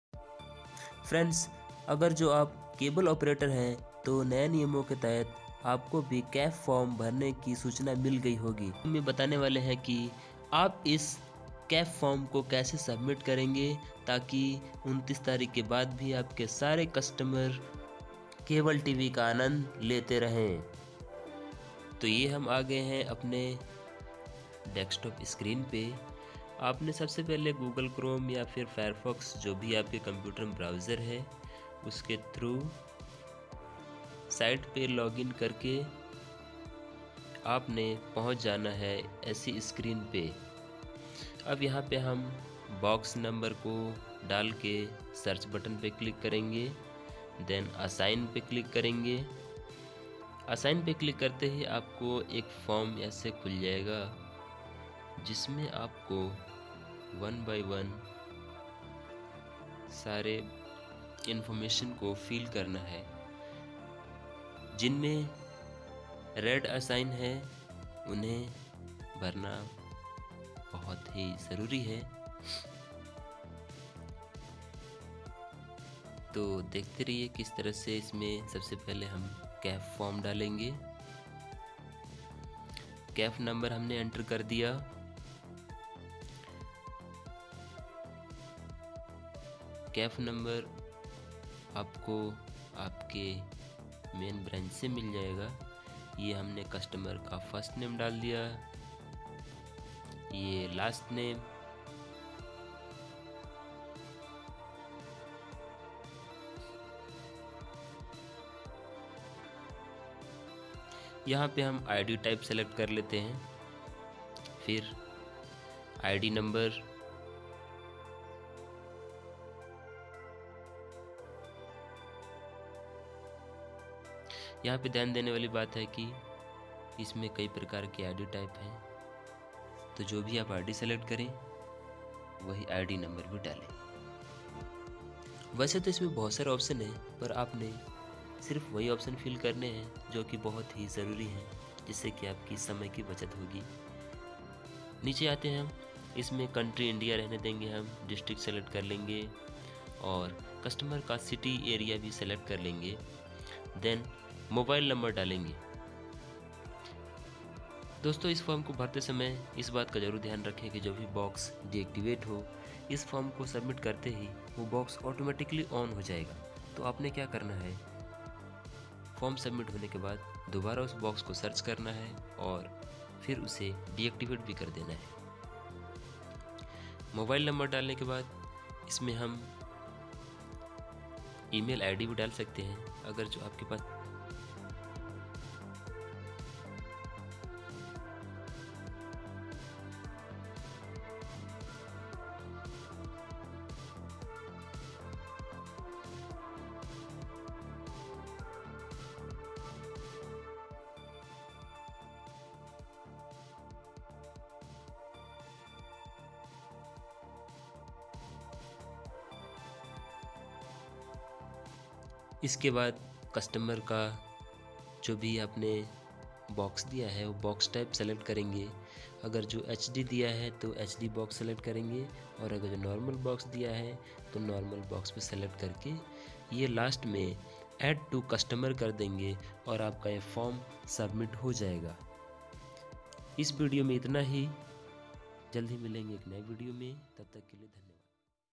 CAF form online kaise bhare | How to fill up CAF form online cable opraters
This video shows how to fill CAF form online by cable opraters.
Mi 9 smartphone.
nack band
Power bank
Selfi stick
Camere holder
Sliding sidhi
AV cable.
HDMI cable